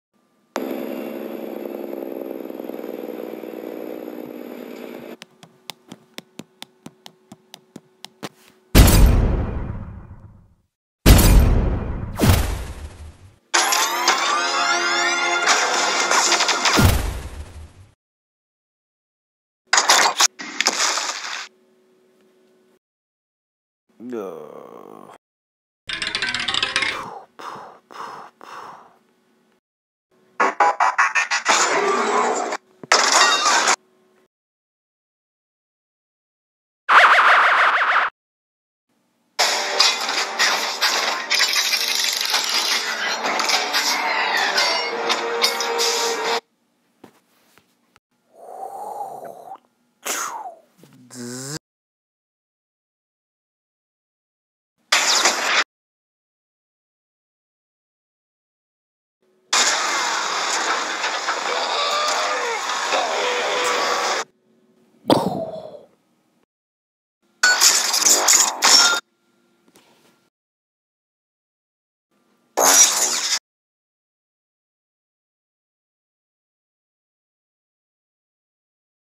Transformers AOE Final Stop Motion!
We'll there you go guys my final stop motion, I worked hard for this took a couple of hours and a lot of editing, the sound may not connect with the things that are happening, but I tried and if you didn't see the last two stop motions,
PS. if you actually read the whole thing and got to this part congrats! Not my best I admit, but still I tried and if some hates, i'm fine with that. PEACE!
Follow me social media to be updated for my future reviews, pick ups and photo gallery.